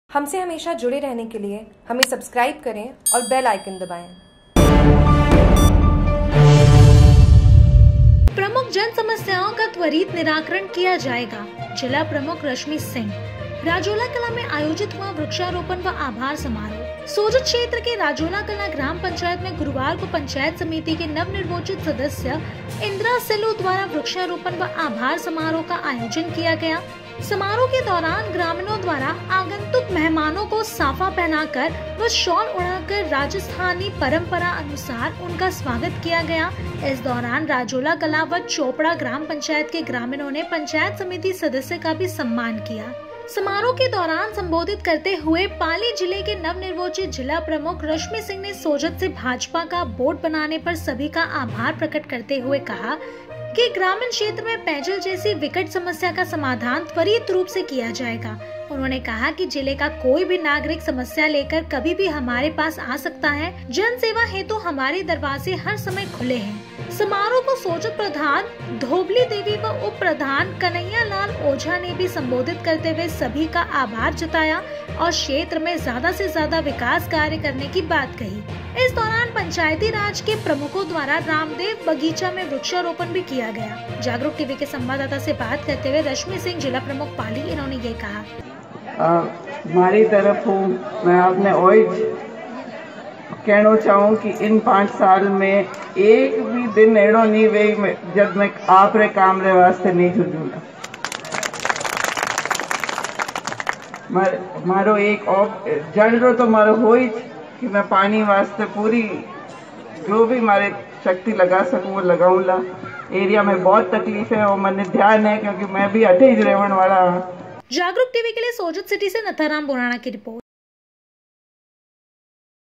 Jagruk TV : प्रमुख जन समस्याओ का त्वरित निराकरण किया जायेगा- जिला प्रमुख रश्मिसिंह
सोजत क्षैत्र के राजोला कलां ग्राम पंचायत मे गुरूवार को पंचायत समिति के नवनिर्वाचित सदस्य इन्द्रा सिलू द्वारा वृक्षारोपण व आभार समारोह का आयोजन किया गया।

समारोह के दौरान ग्रामीणो द्वारा आगन्तुक मेहमानो को साफा पहनाकर व शालॅ ओढाकर राजस्थानी परम्परानुसार उनका स्वागत किया गया।इस दौरान राजोला कलां व चौपड़ा ग्राम पंचायत के ग्रामीणो ने पंचायत समिति सदस्य का भी सम्मान किया।

समारोह के दौरान संबोधित करते हुए पाली जिले के नवनिर्वाचित जिला प्रमुख रश्मिसिंह ने सोजत मे भाजपा का बोर्ड बनाने पर सभी का आभार प्रकट करते हुए कहा कि ग्रामीण क्षेत्र मे पेयजल जैसी विकट समस्या का समाधान त्वरित रूप से किया जायेगा।उन्होने कहा कि जिले का कोई भी नागरिक समस्या लेकर कभी भी हमारे पास आ सकता है जनसेवा हेतु हमारे दरवाजे हर समय खुले है।

समारोह को सोजत प्रधान धोबलीदेवी व उप-प्रधान कन्हैयालाल औझा ने भी संबोधित करते हुए सभी का आभार जताया और क्षेत्र मे ज्यादा से ज्यादा विकास कार्य करने की बात कही। इस दौरान पंचायतीराज के प्रमुखो द्वारा रामदेव बगीची मे वृक्षारोपण भी किया गया।

बाईट- रश्मिसिंह,जिला प्रमुख पाली।

रिपोर्ट- नथाराम बौराणा